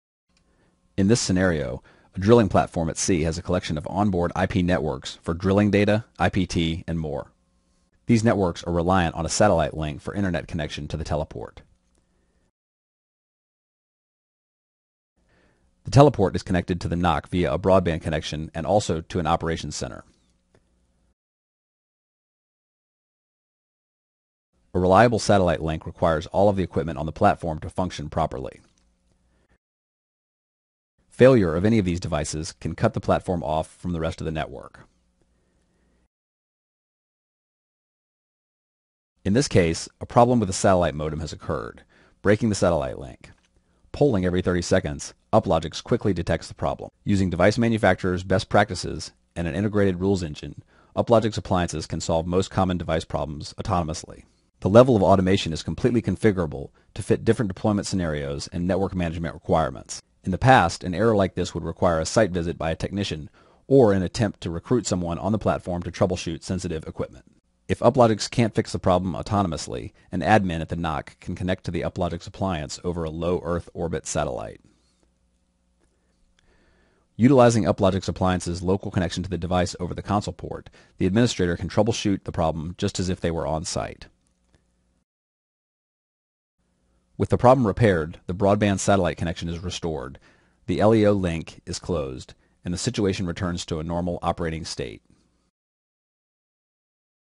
Uplogix deployment in a maritime VSAT network
Uplogix appliances automate most network management tasks 24x7 and enable operators to remotely access, monitor, and control both satellite and traditional networking equipment.